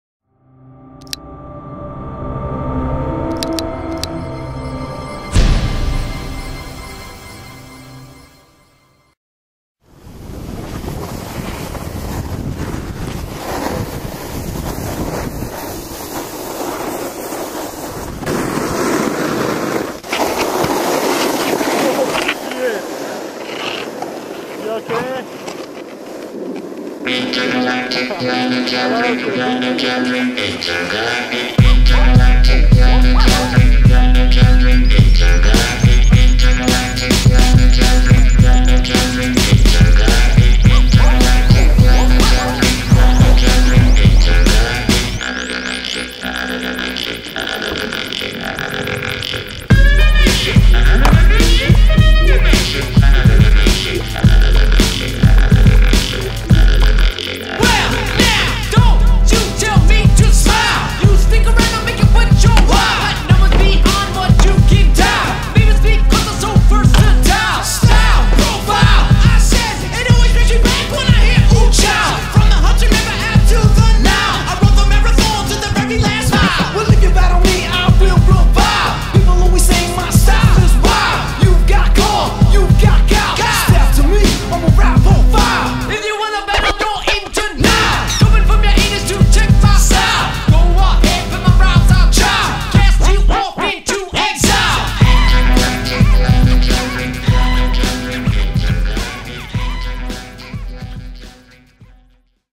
Trevor Cowen "Touchy Feely" Promo | ExperimentalDesign
Trevor Cowen shredding Trollhaugen in Dresser, WI on a chilly Friday night.

Look for the full video soon!